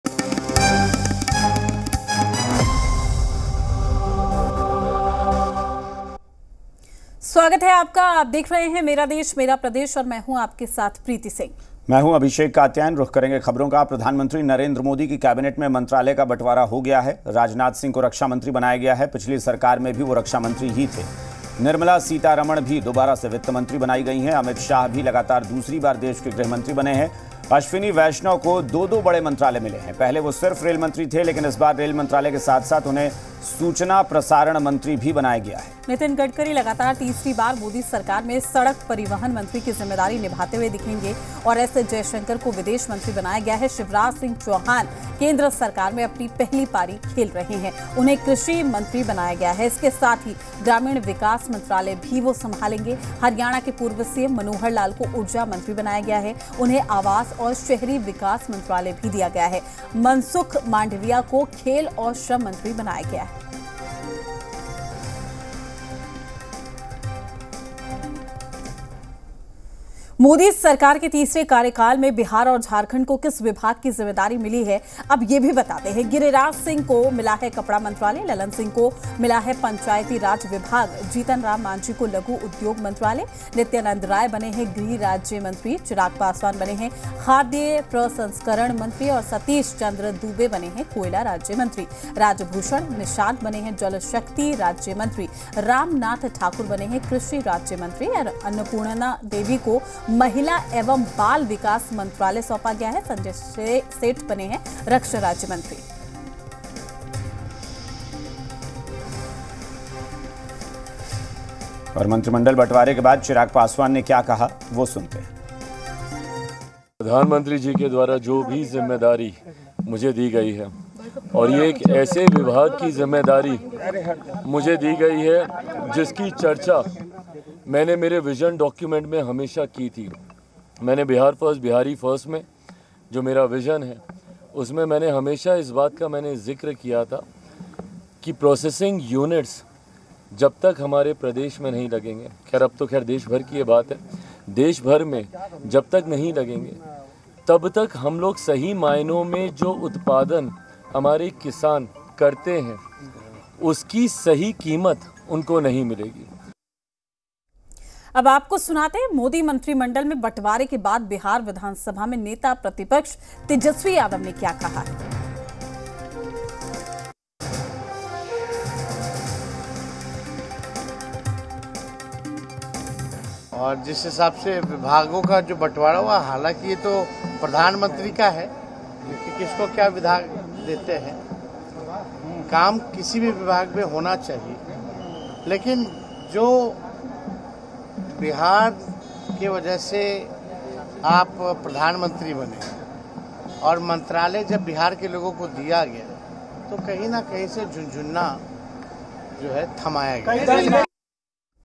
PM Modi Cabinet Portfolio: बिहार के किस मंत्री को मिला कौन सा विभाग ? | Modi 3.0
PM Modi Cabinet Portfolio मोदी सरकार के तीसरे कार्यकाल में बिहार और झारखंड को किस विभाग की जिम्मेदारी मिली है...अब ये भी बताते हैं...
-गिरिराज सिंह को मिला कपड़ा मंत्रालय
-ललन सिंह को मिला पंचायती राज विभाग
-जीतनराम मांझी को लघु उद्योग मंत्रालय
-नित्यानंद राय बने गृह राज्य मंत्री
-चिराग पासवान बने खाद्य प्रसंस्करण मंत्री
-सतीश चंद्र दुबे बने कोयला राज्य मंत्री
-राजभूषण निषाद बने जलशक्ति राज्य मंत्री
-रामनाथ ठाकुर बने कृषि राज्य मंत्री
-अन्नपूर्णा देवी को महिला एवं बाल विकास मंत्रालय
-संजय सेठ बने रक्षा राज्य मंत्री